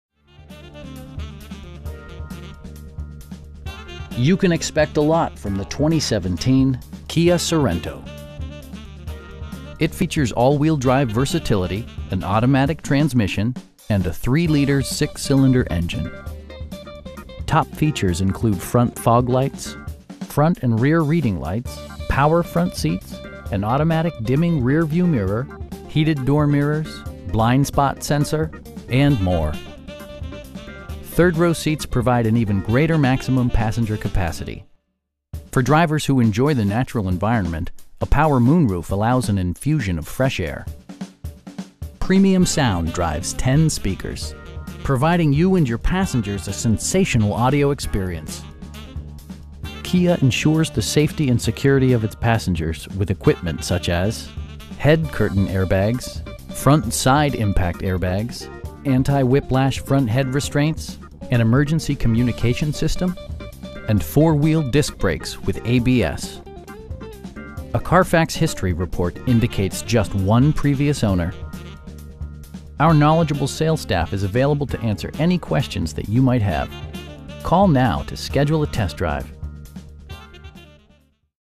2017 Kia Sorento 3.3L SX in Wilmington, DE 19806
You can expect a lot from the 2017 Kia Sorento. It features all-wheel drive versatility, an automatic transmission, and a 3 liter 6 cylinder engine. Top features include front fog lights, front and rear reading lights, power front seats, an automatic dimming rear-view mirror, heated door mirrors, blind spot sensor, an overhead console, and more. Features such as automatic climate control and leather upholstery prove that economical transportation does not need to be sparsely equipped. Third row seats provide an even greater maximum passenger capacity. For drivers who enjoy the natural environment, a power moon roof allows an infusion of fresh air. Premium sound drives 10 speakers, providing you and your passengers a sensational audio experience. Kia ensures the safety and security of its passengers with equipment such as: head curtain airbags, front side impact airbags, traction control, brake assist, anti-whiplash front head restraints, ignition disabling, an emergency communication system, and 4 wheel disc brakes with ABS. For added security, dynamic Stability Control supplements the drivetrain. A Carfax history report indicates just one previous owner. Our knowledgeable sales staff is available to answer any questions that you might have. They'll work with you to find the right vehicle at a price you can afford.